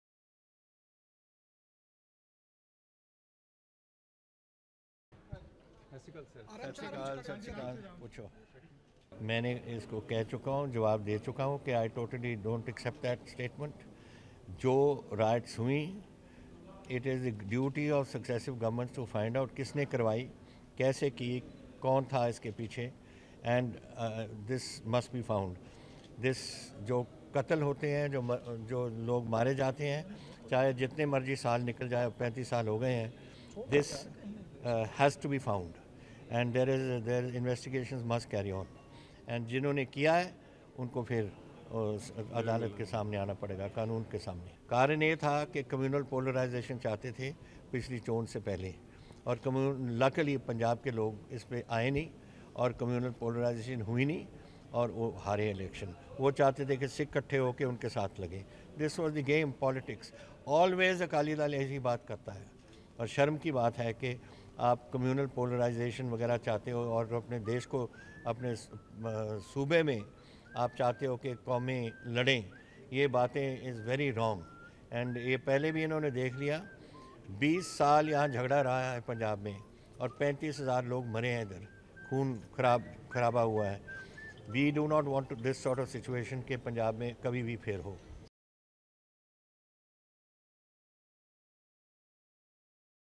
It is duty of successive govt’s to find out how 1984 riots happened: Captain Amarinder Singh
Jalandhar (Punjab), May 10 (ANI): Amidst the Lok Sabha elections, while speaking to ANI on Indian Overseas Congress Chief Sam Pitroda’s remark on 1984 anti-Sikh riots, Punjab Chief Minister Captain Amarinder Singh said